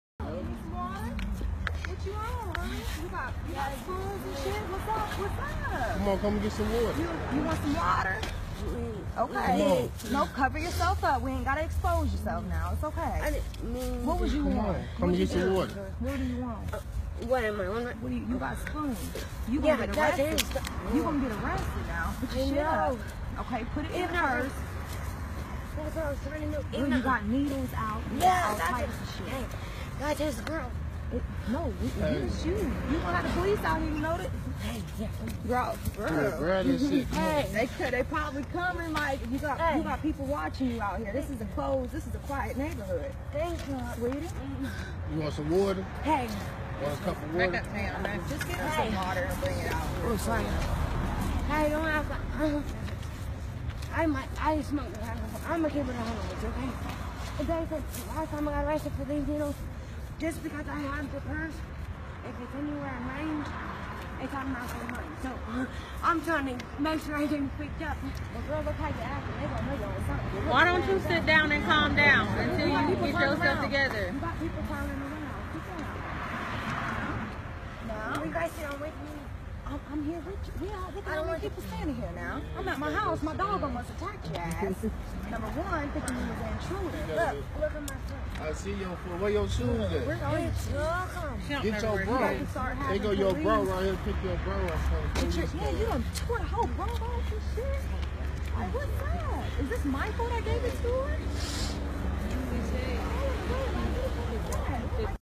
HONOKAA HAWAII, JERKWATER U.S.A. WOMAN ON METH
HONOKAA WOMAN STRUNG OUT ON CRACK OR METH.